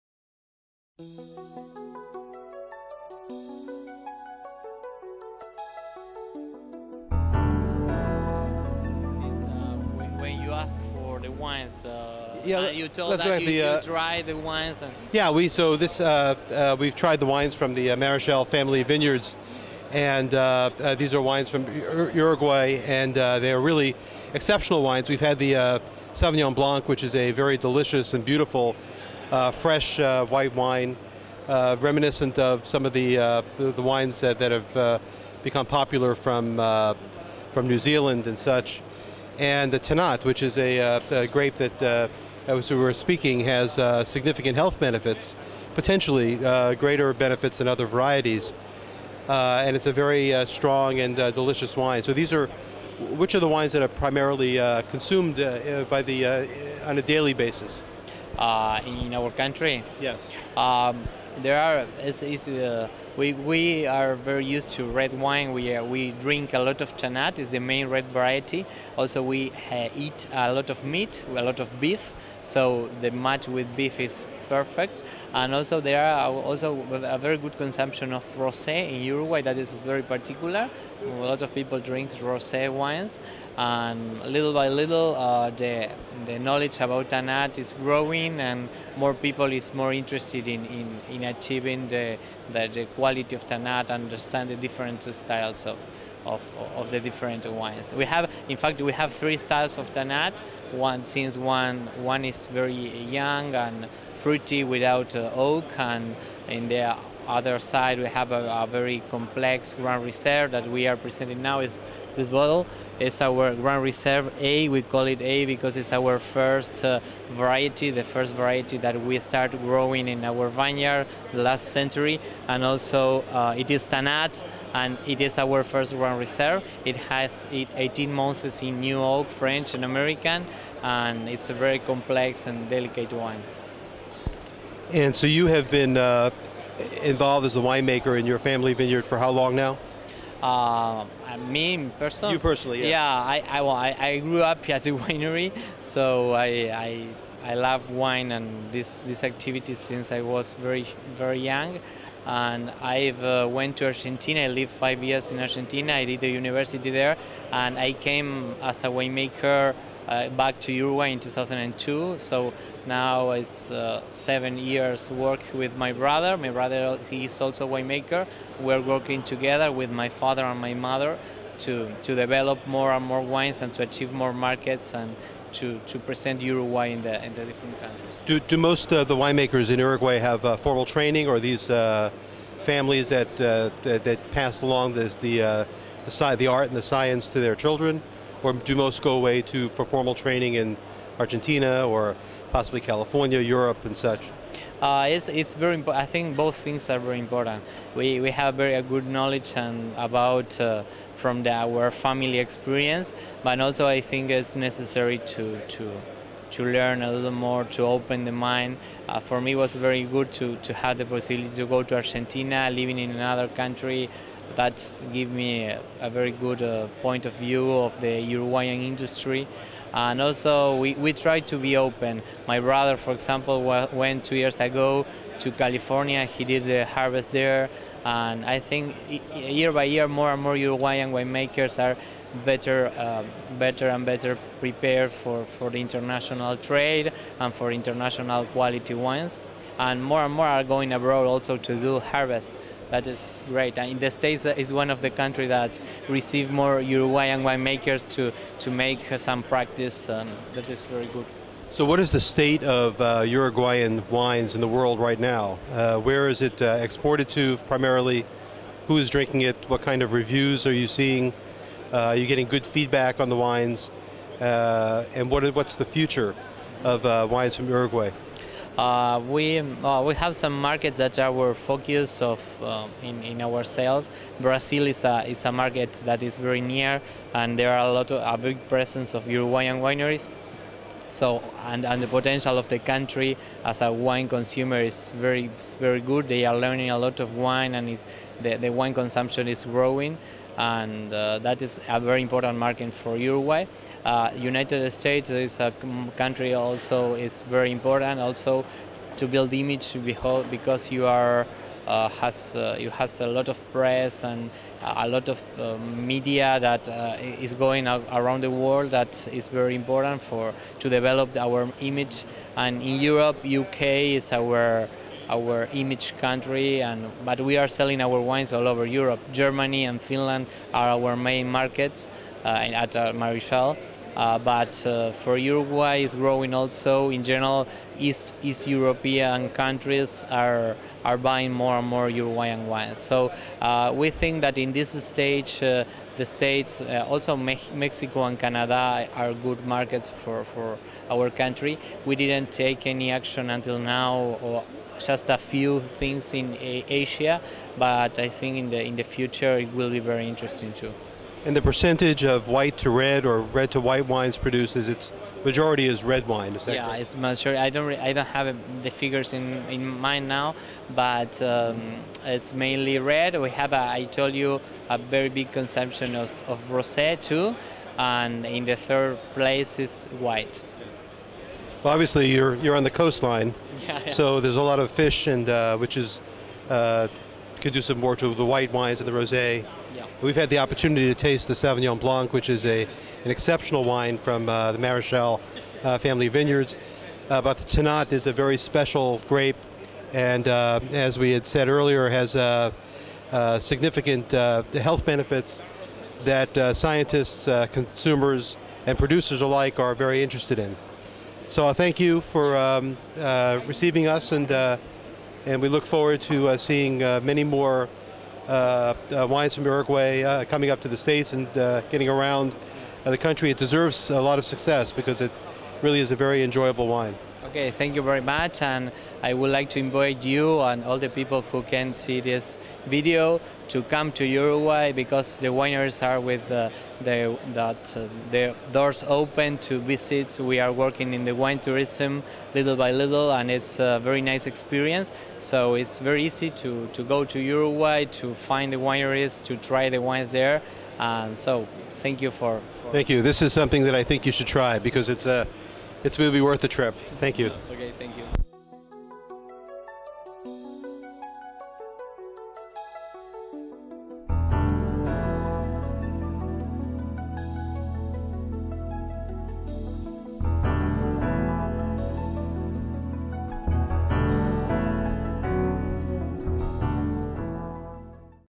Interview with Juan Marichal.m4v
An interview with winemaker Juan Marichal, Bodega Marichal, Uruguay, recorded in Bordeaux, France, 2009.
This interview was produced by wineMDonline.com a resource for wine and health information for physicians and consumers.
wineMDonline.com is scheduled for launch 2010.